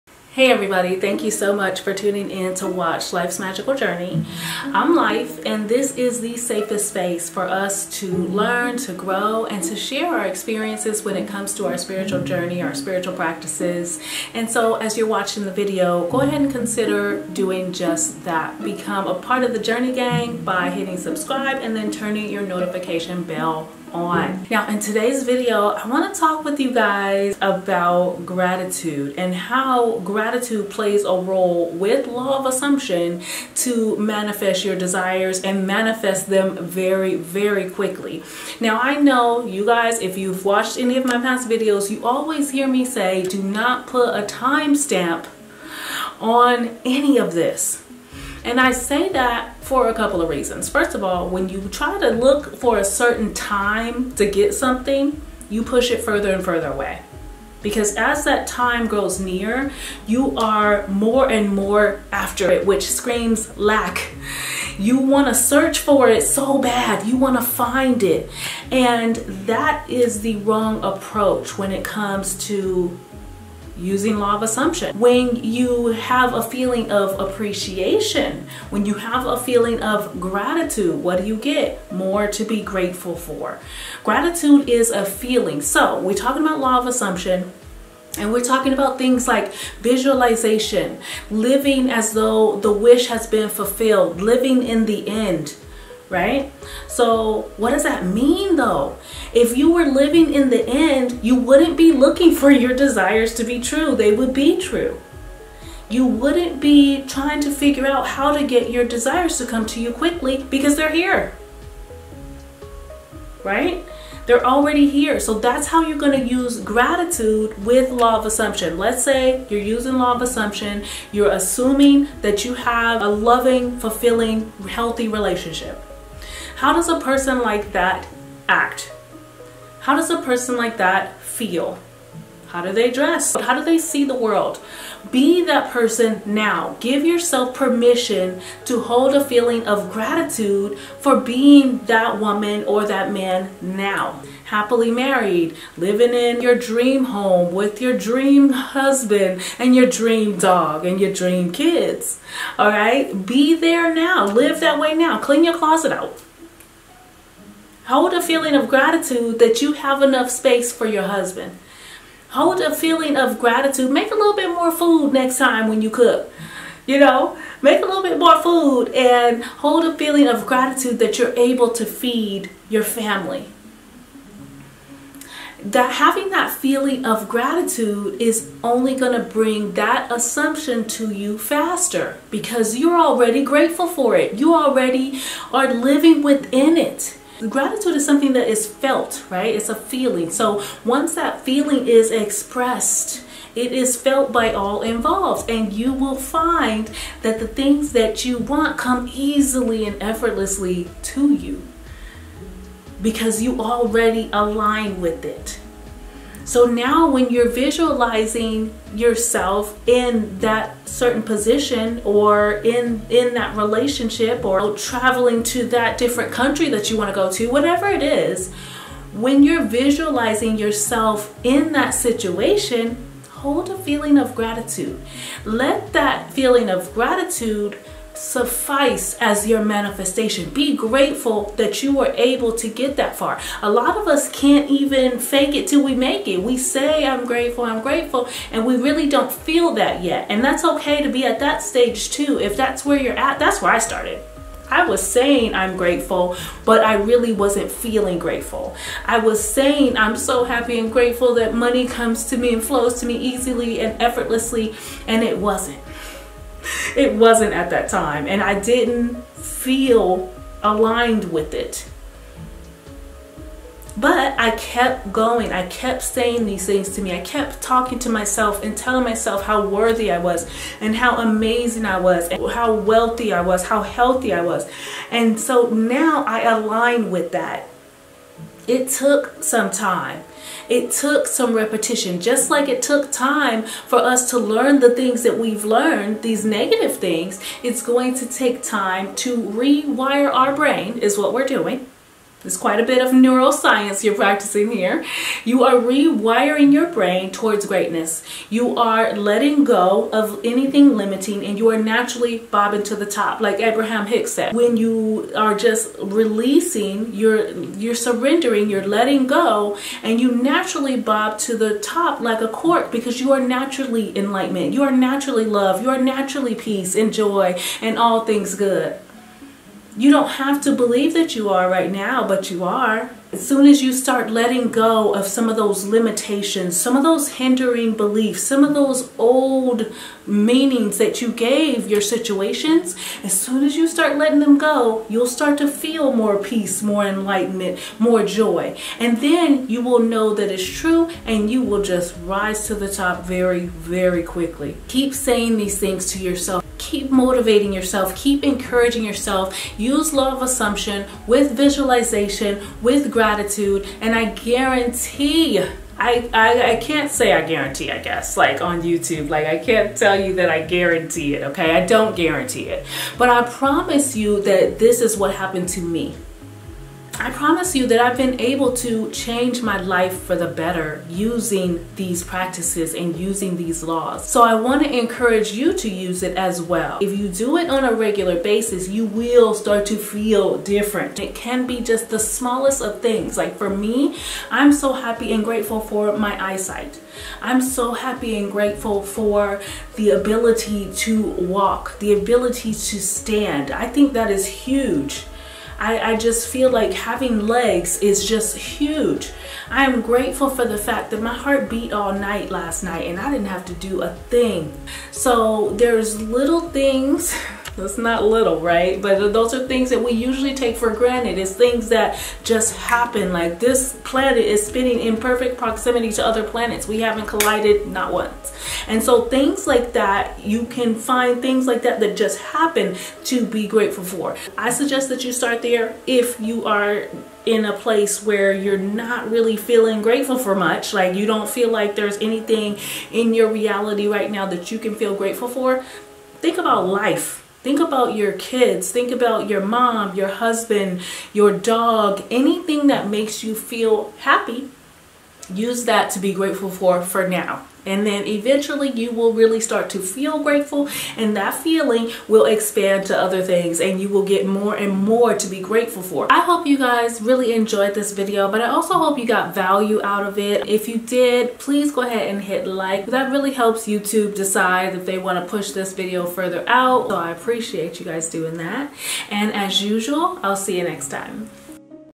We really do need gratitude to use Law of Assumption! Gratitude and Law of Assumption explained
We really do need gratitude to be able to purposely use Law of Assumption! Gratitude and Law of Assumption explained. Create deliberately!

Law of Assumption and Gratitude?! Do they really go hand in hand? Do we really need gratitude to be able to use Law of Assumption deliberately? I mean they say that gratitude changes everything but are they on to something? Do we need gratitude or is that just another fairytale?

Will holding gratitude in your heart make or break your manifestations? In a nutshell the answer is yes. Gratitude is a feeling. Gratitude, like all other things, is a vibration. If you sent of a vibration of gratitude, you get more to be grateful for.

 Saying that you are grateful and at least attempting to hold the feeling of gratitude in your heart will definitely bring a feeling of deep appreciation that will be undeniable. Gratitude is something that can be easily felt by others as well. Usually if someone is really grateful for something that feeling is spread out and anyone involved usually can feel the gratitude coming from them.

Using Law of Assumption along with gratitude is one of the most effective ways to use Law of Assumption and create your reality! You'll find that creating your own reality is easy when you simply keep gratitude at the forefront of your heart and mind. Law of Assumption will surely assist and bring your desires to you very quickly..

So yes, we really do need gratitude while using the Law of Assumption. Without gratitude, things just won't work out how they're suppose to or thing won't last very long. Gratitude has to be on the top of the list of things to do while creating deliberately using Law of Assumption.

When you pair Law of Assumption with gratitude you are sure to get explosive results! You are sure to see your manifestations come to you more and more quickly once you're able to use Gratitude and Law of Assumption purposely!